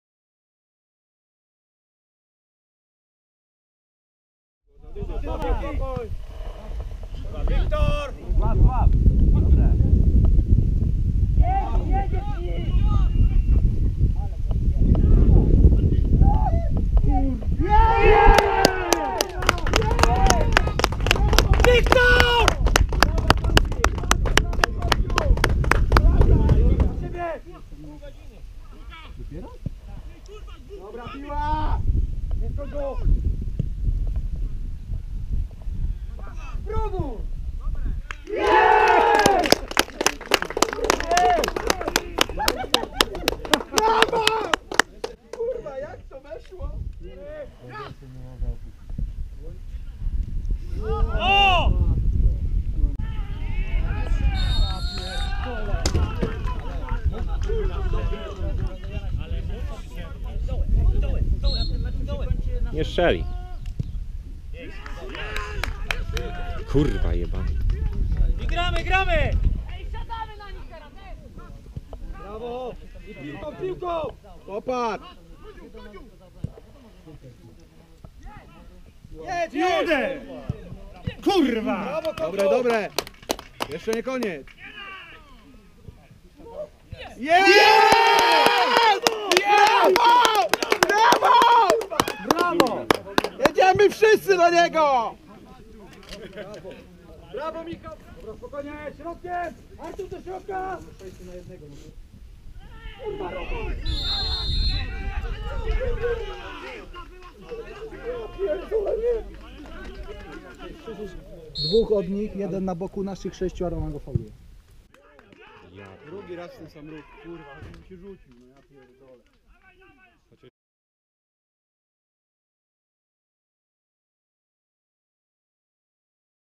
RKS Radliczyce 3:2 LZS Godziesze
Bramki z 6 kolejki kaliskiej B-klasy w sezonie 16/17.